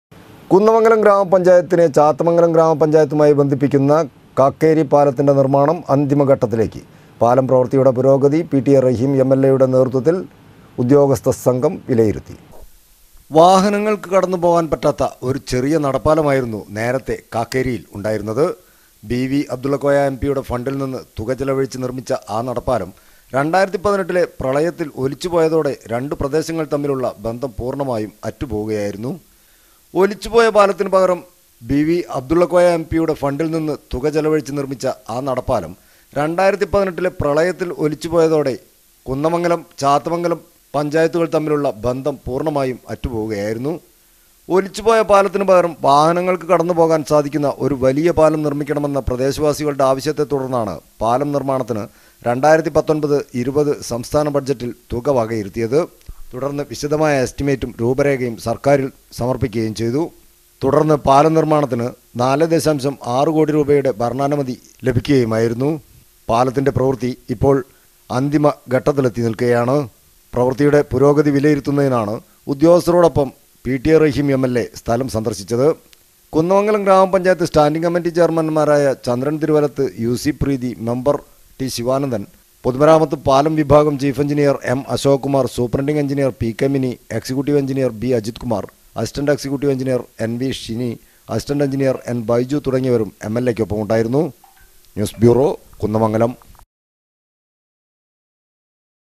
ചാത്തമംഗലം ഗ്രാമപഞ്ചായത്തുമായി ബന്ധിപ്പിക്കുന്ന കാക്കേരി പാലത്തിന്റെ നിർമ്മാണം അന്തിമ ഘട്ടത്തിലേക്ക്
കുന്ദമംഗലം ഗ്രാമപഞ്ചായത്തിനെ ചാത്തമംഗലം ഗ്രാമപഞ്ചായത്തുമായി ബന്ധിപ്പിക്കുന്ന കാക്കേരി പാലത്തിന്റെ നിർമ്മാണം അന്തിമ ഘട്ടത്തിലേക്ക്. പാലം പ്രവൃത്തിയുടെ പുരോഗതി പി.ടി.എ റഹീം എം.എൽ.എയുടെ നേതൃത്വത്തിൽ ഉദ്യോഗസ്ഥ സംഘം വിലയിരുത്തി.